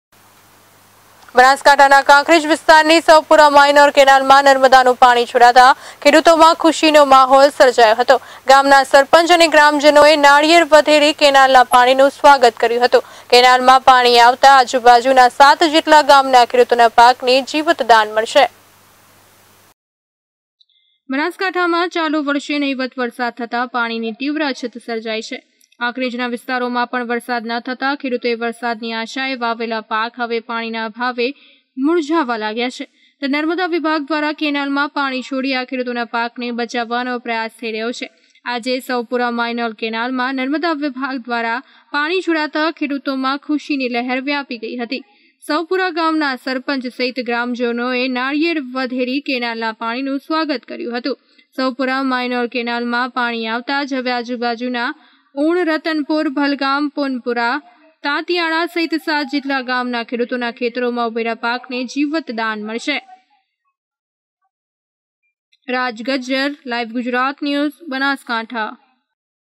બનાસકાંઠા: સવપુરા માઇનોર કેનાલમાં નર્મદાનું પાણી છોડાતા ખેડૂતોમાં ખુશીનો માહોલ
બનાસકાંઠાના કાંકરેજ વિસ્તારની સવપુરા માઇનોર કેનાલમાં નર્મદાનું પાણી છોડાતા ખેડૂતોમાં ખુશીનો માહોલ સર્જાયો હતો. ગામના સરપંચ અને ગ્રામજનોએ નાળિયેર વધેરી કેનાલના પાણીનું સ્વાગત કર્યું હતું .કેનાલમાં પાણી આવતાં આજુબાજુના સાત જેટલા ગામના ખેડૂતોના પાકને જીવતદાન મળશે..